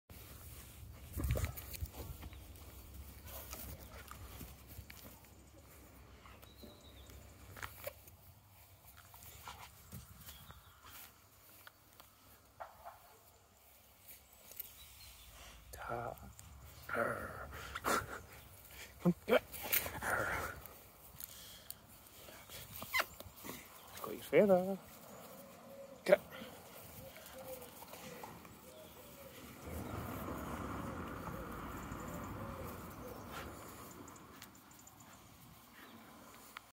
Max the Springer Spaniel Dog Having Fun With A Feather!
Here we have Max the 5 year old English Springer Spaniel dog, with yet another of his favourite things, this time a feather.
He is easily amused like all springer spaniels can be, I could him messing around all day!

Thank you, Darren.